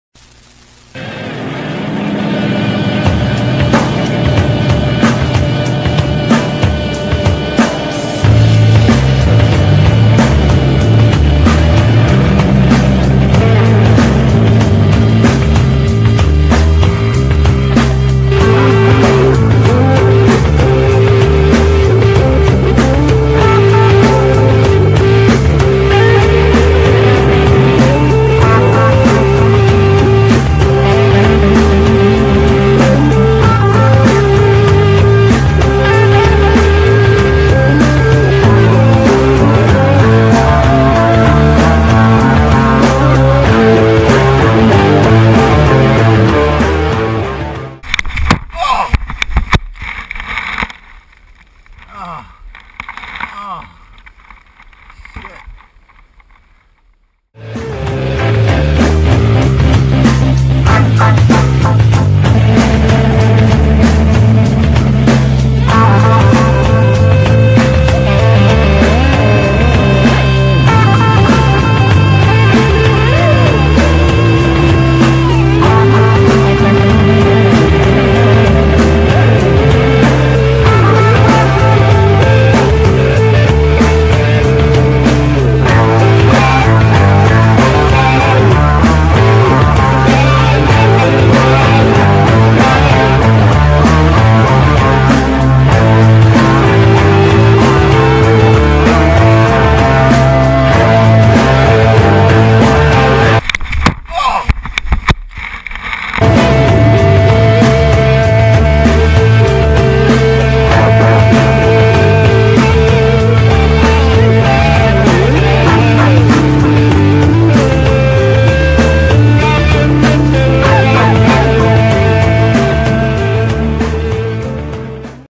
POV Mtb Bike Tree Face Plant
Oak Tree-1 Me-0. Rough finish to a good day of Mountain Biking on Round Mountain Trail, Nevada City, CA